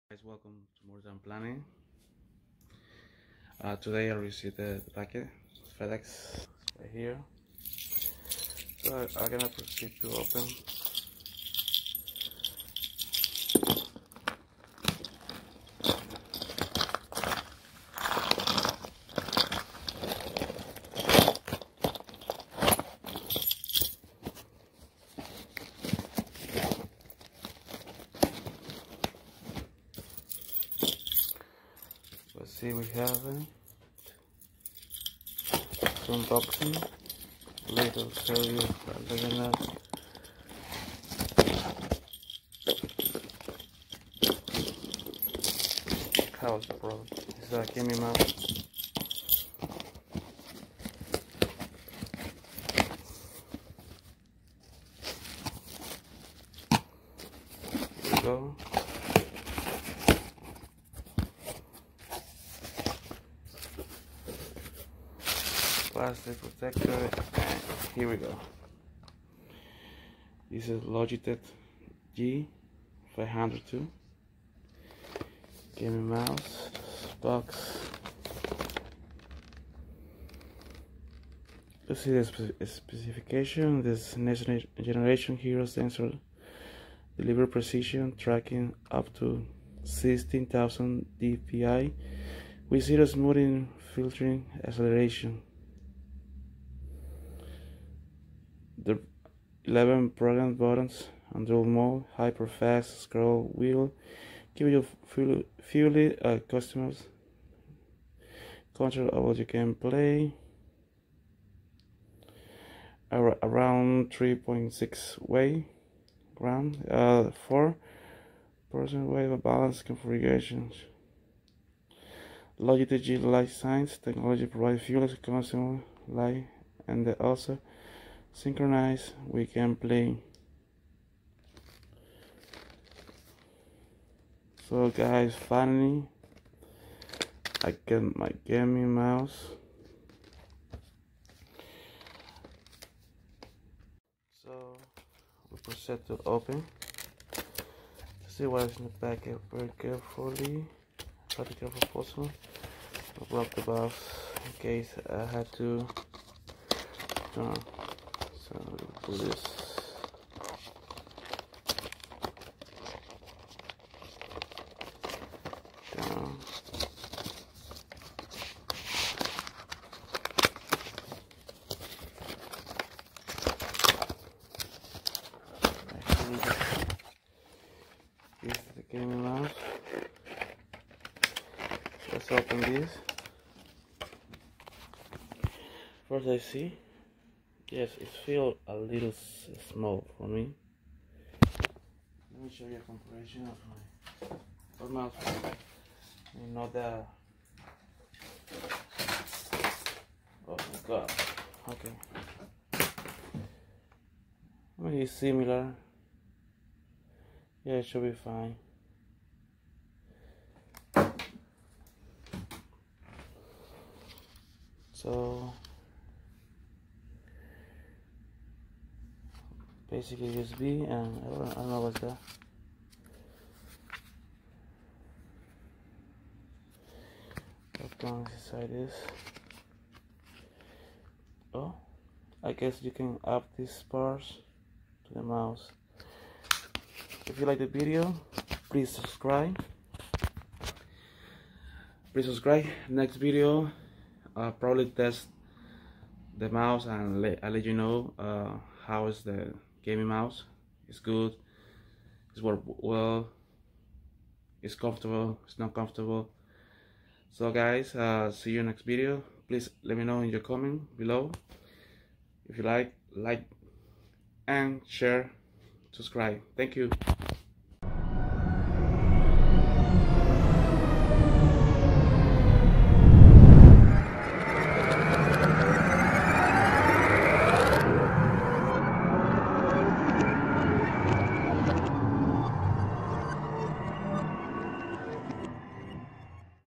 My first G 502 logitech hero gaming mouse unboxing
My first G 502 logitech gaming mouse  unboxing  2020

this product i buy for my personal used